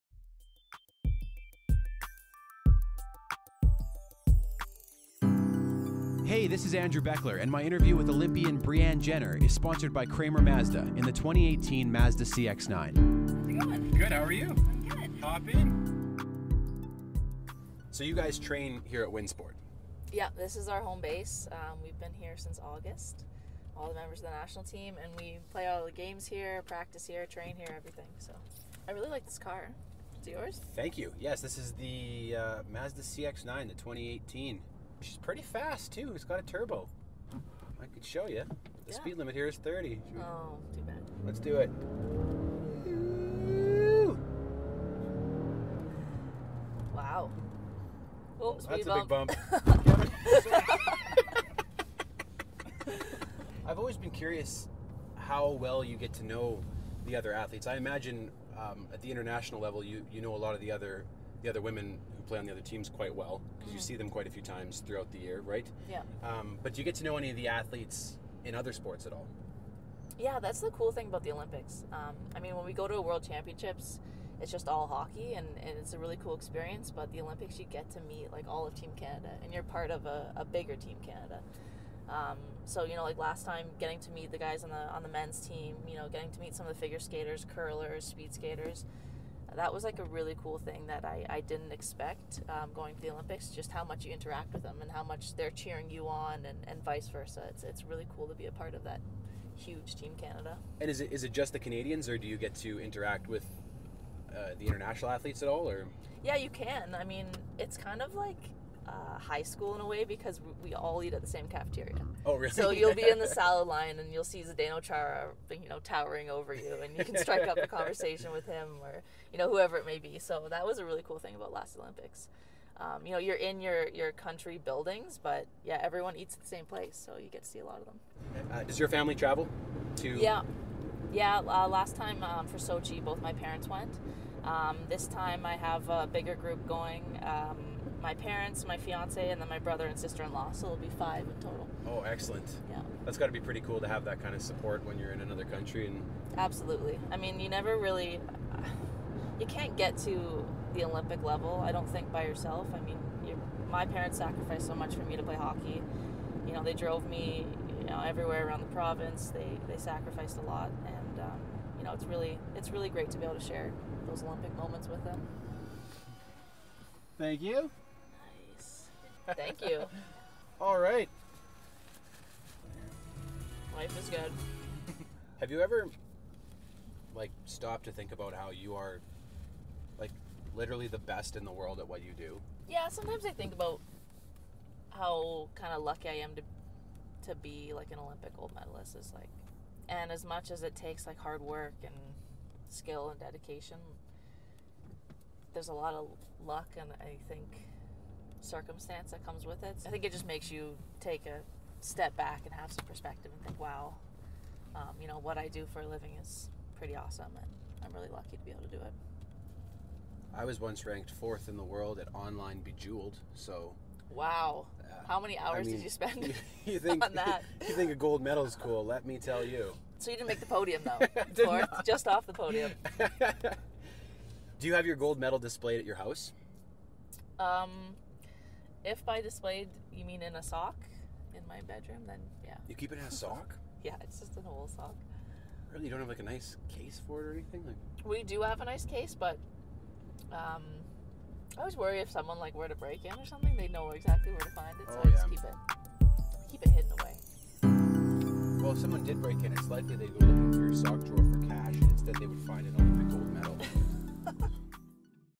Exclusive Interview With Olympian Brianne Jenner of the Canadian Women's Hockey Team - Kramer Mazda
Check out Kramer Mazda's exclusive interview with Olympian Brianne Jenner of the Canadian Women's Hockey Team!

And don't forget to cheer her on next month as she competes for gold in South Korea!